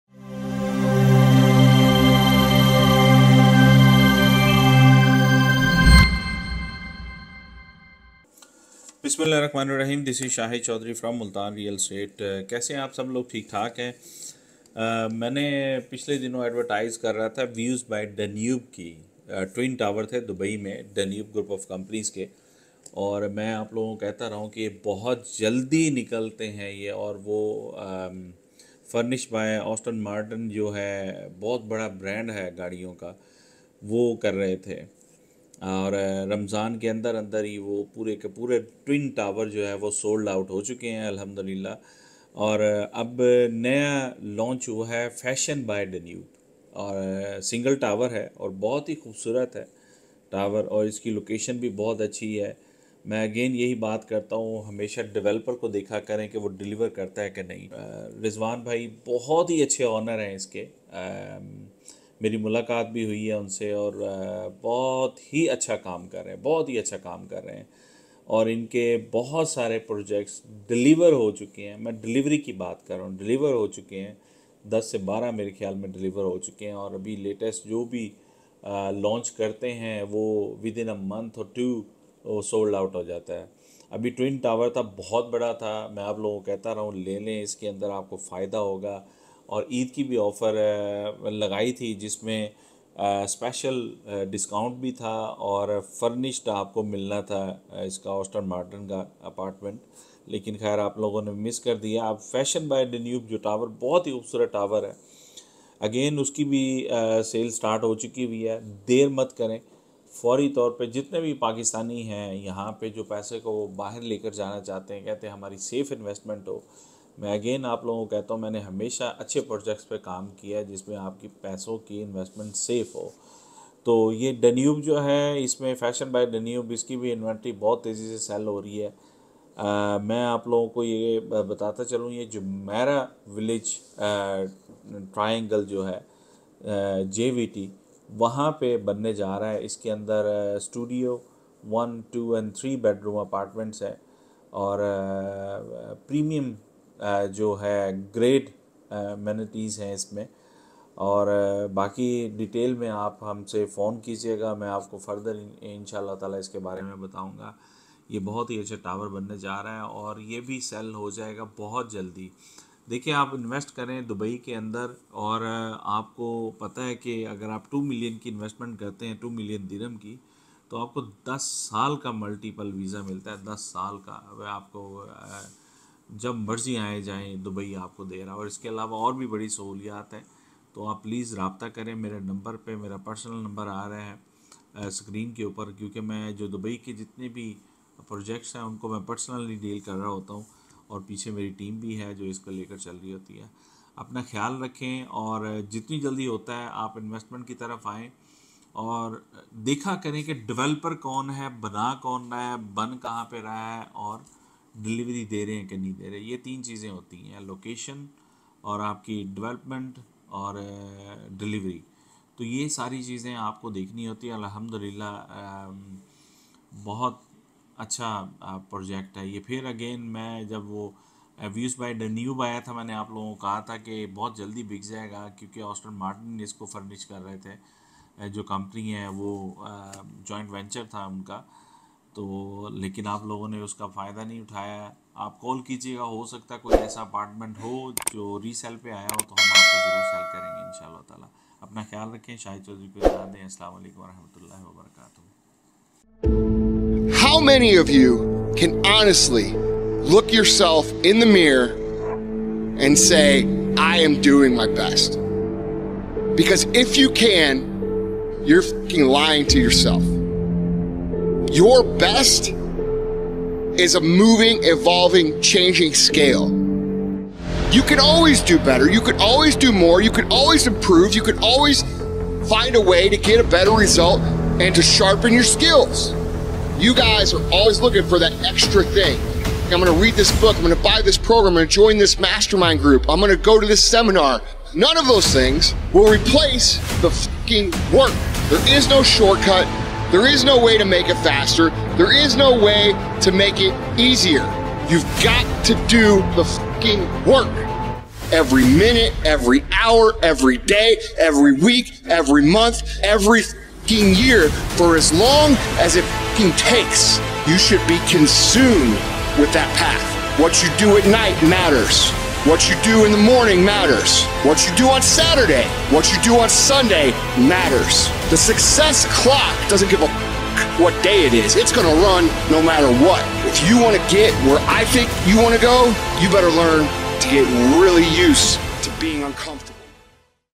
Fashionz By Danube at JVT | Dubai's First Fashion TV Branded Residences
Fashionz By Danube - Dubai's First FashionTV branded residences at Jumeirah Village Triangle (JVT), Dubai by Danube Properties - Choose from studio,1, 2, and 3 bedroom fully furnished apartments in JVT, starting from AED 850,000. FASHIONZ brings you luxury, comfort, and style by blending indoor and outdoor amenities inspired by style icons

Dubai’s first FashionTV Branded Residences FASHIONZ - A Haute Couture lifestyle, where individuality is celebrated and fashion comes alive every day. Fall head over heels as you experience luxury living at FASHIONZ, one of the tallest towers in Jumeirah Village Triangle,
Inspired by style icons, FASHIONZ is fashionably modern with a perfect blend of indoor and outdoor amenities to bring you a world of comfort, luxury, and style

✅ 1% DownPayment Plan
✅ Bespoke Living
✅ Fashion TV Themed Spaces
✅ Beautifully Crafted Luxurious Homes

✅ Features & Amenities
-Private Pool
-Parking Space
-Gardens
-Sports Courts
-Gym
-Running Tracks
-BBQ Areas
-Parks and Gardens

✅ Connectivity
-Burj Khalifa - 20 Minutes
-Downtown Dubai - 20 Minutes
-Dubai International Airport - 25 Minutes
-Dubai Marina - 15 Minutes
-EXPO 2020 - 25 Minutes
-Jumeirah Beach Residence - 15 Minutes
-Mall of the Emirates - 10 Minutes

✅ Starts From AED 850,000

Shahid Choudhry Chairman MRE Group of Companies

We Are Committed To Deliver A Better Service🙌